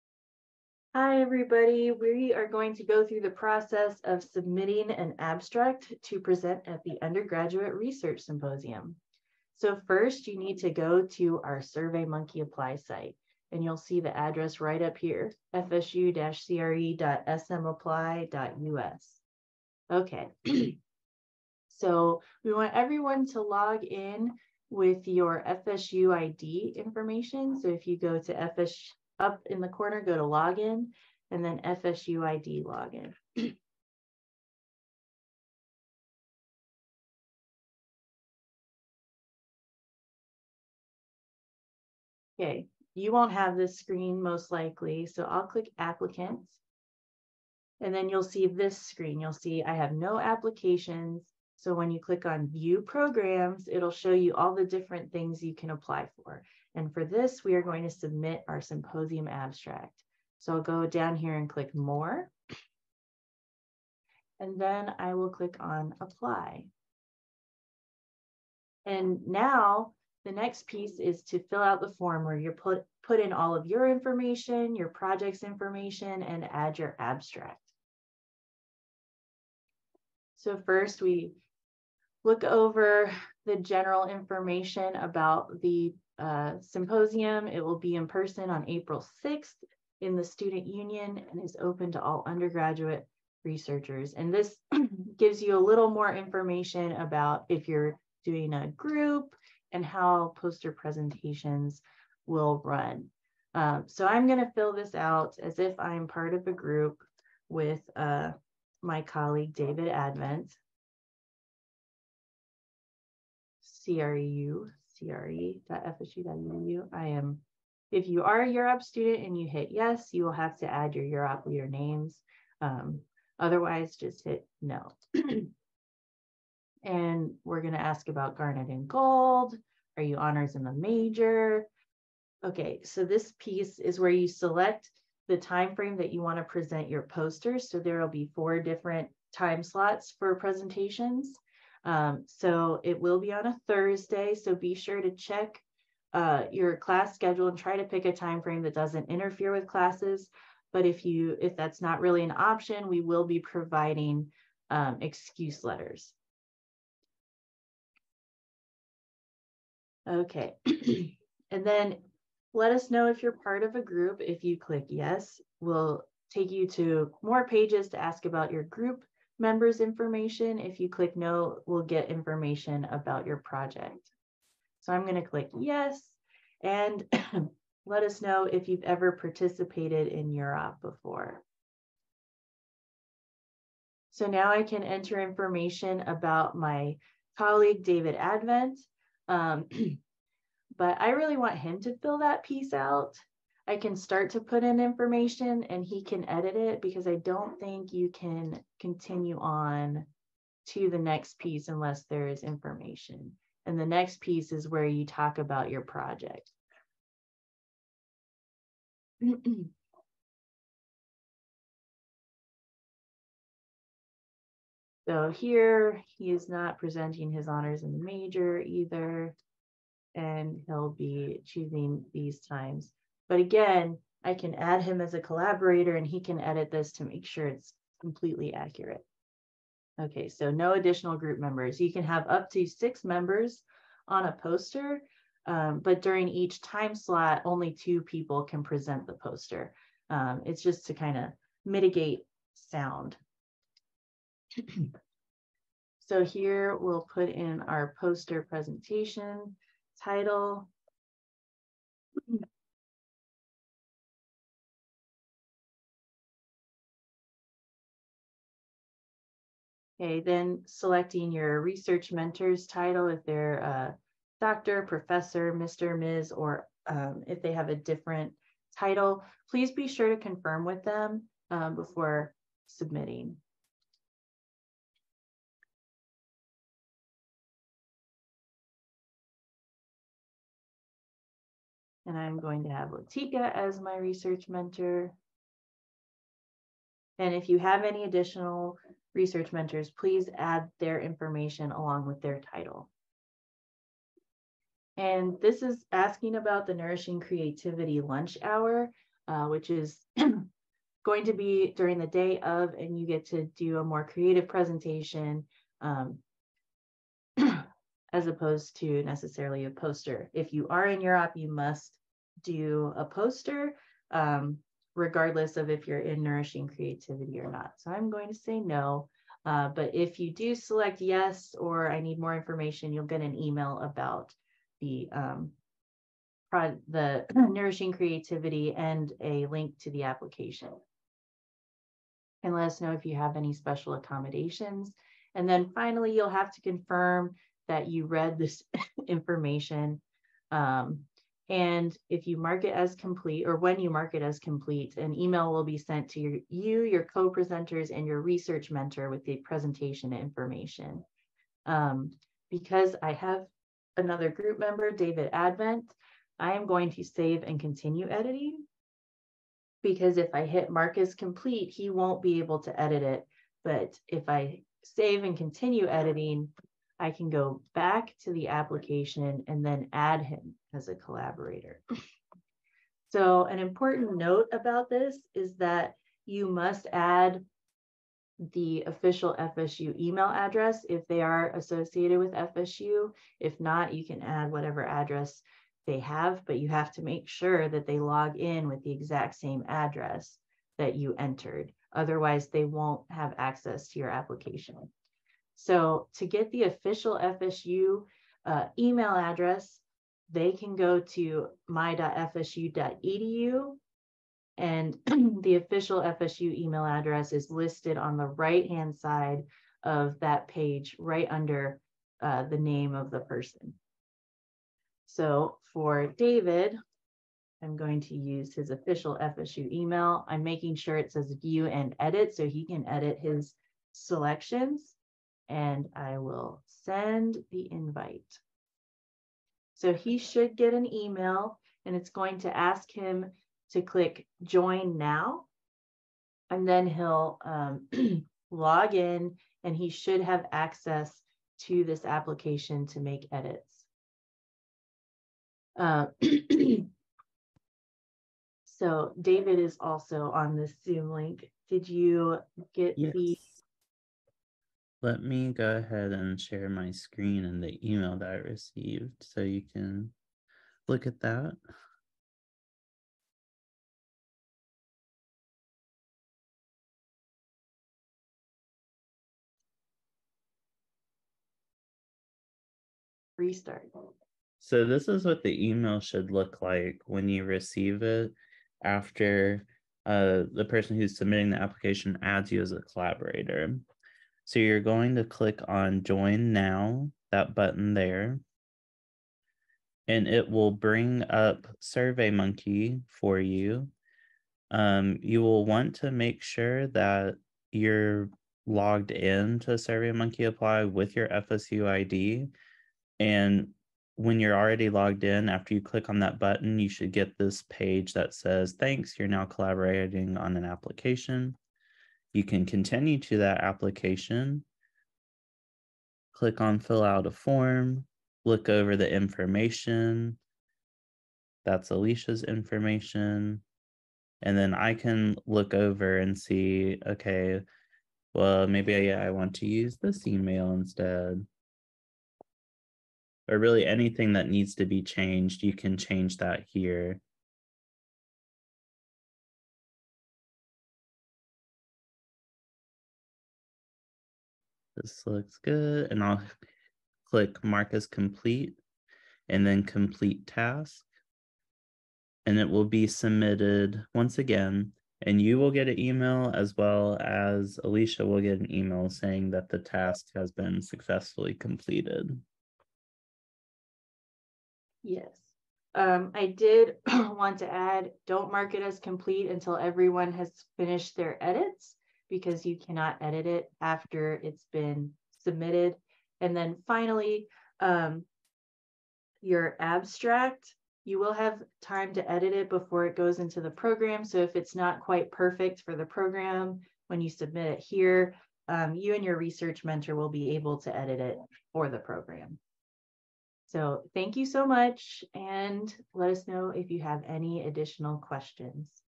Submitting URS 2023 Application
This tutorial provides a step by step guide to submitting your Undergraduate Research Symposium application!